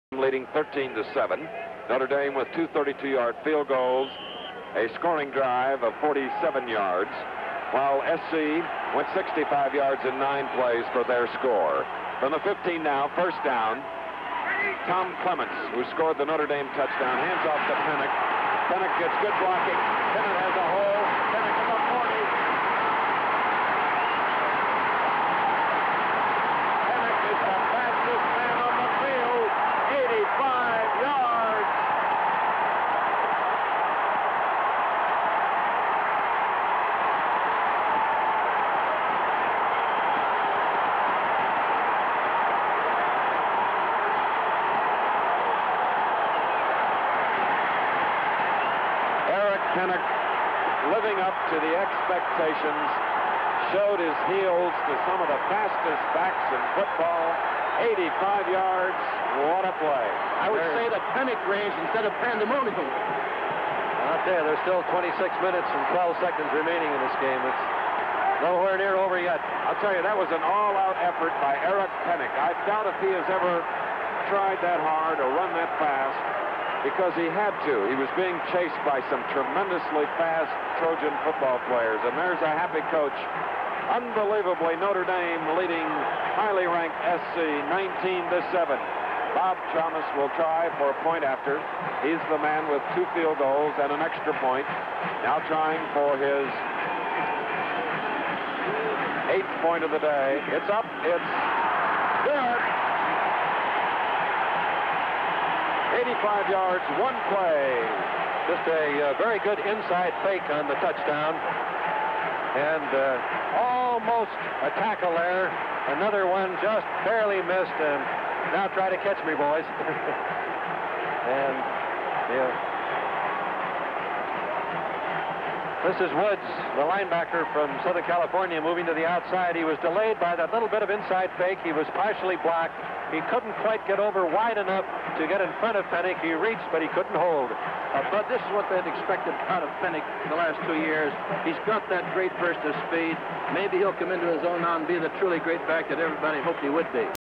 Penick TD 1973 USC vs. Notre Dame
Eric Penick's pivotal 85-yard touchdown run for Notre Dame against USC in 1973. Chris Schenkel, Bud Wilkinson, and Duffy Daugherty called the game on national television.